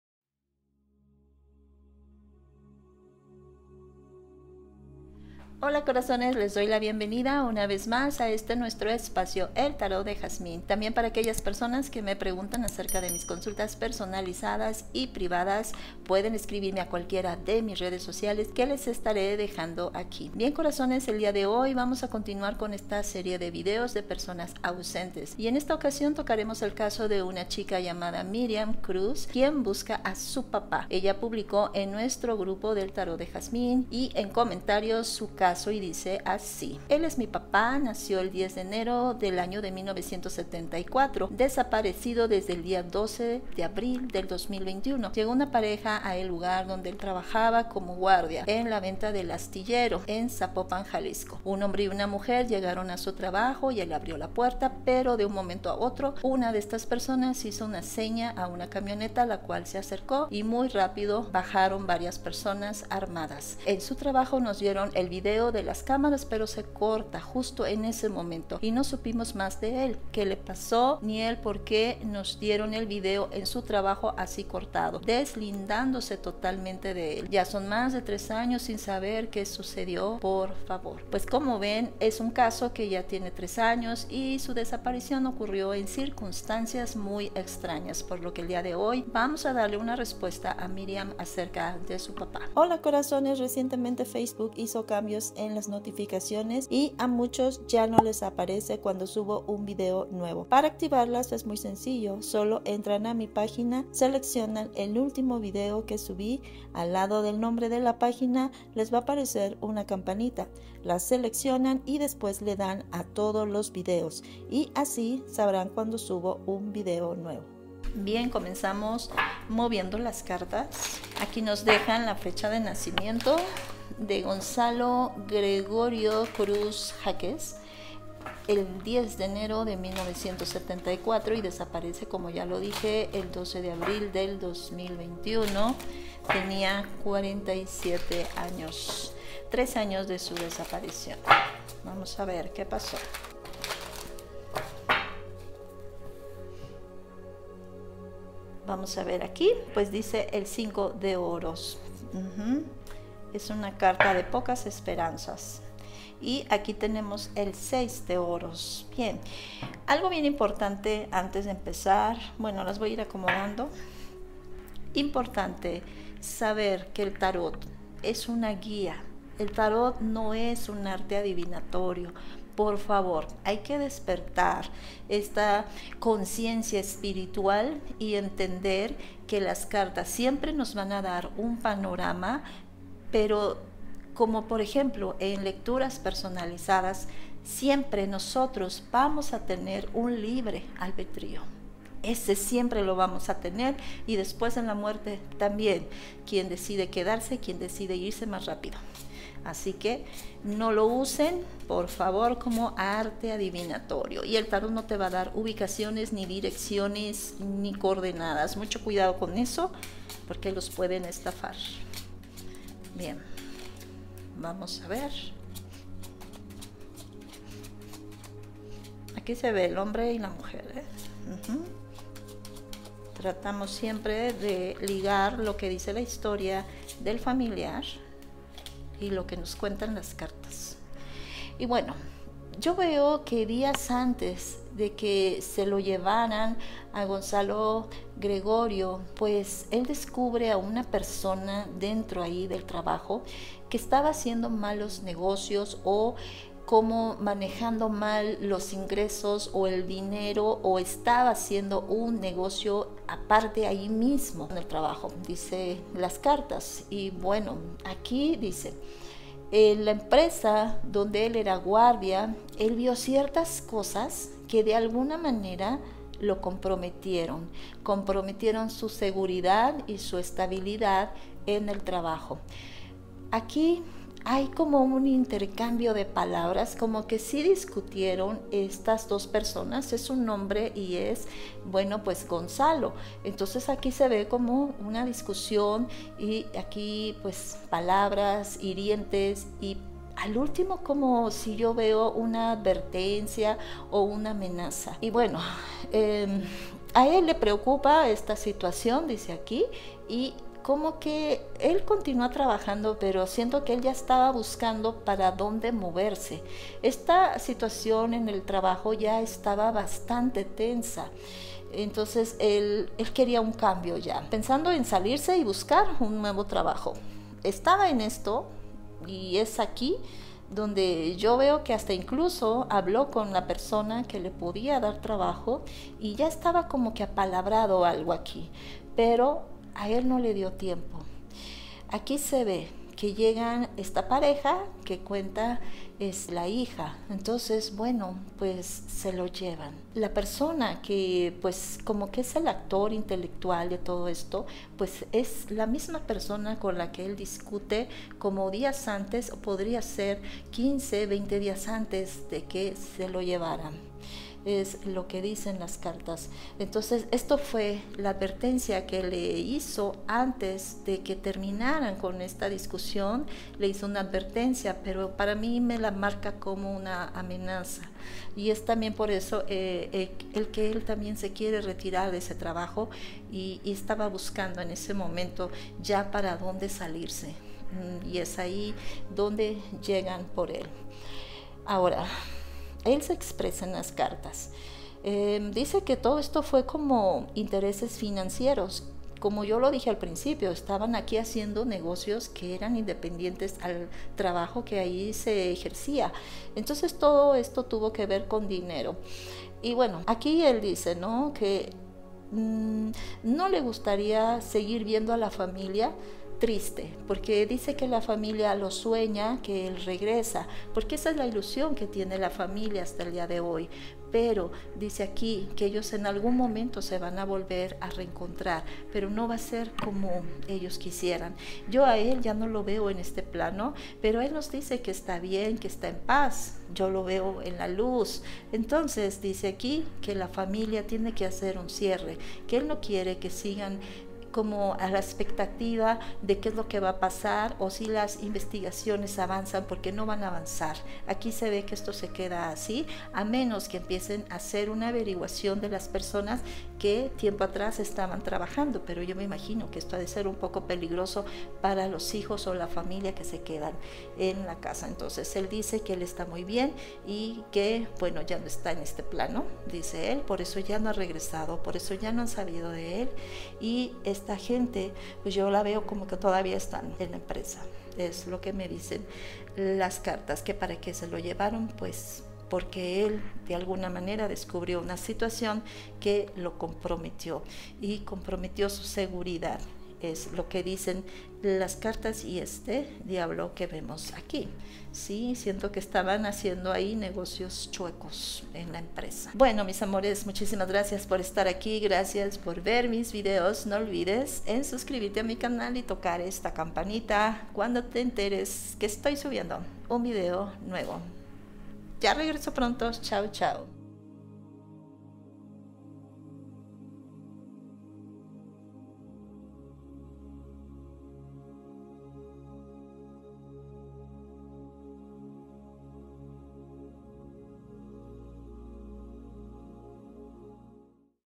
¿Qué pasó con Gonzalo Gregorio Cruz Jaquez? | El Tarot Responde
En este vídeo consulto con el Tarot qué pasó con Gonzalo Gregorio Cruz Jaquez.

Este vídeo lo hago con mucho respeto.

Información de consultas personalizadas:
Facebook: El Tarot de Jazmín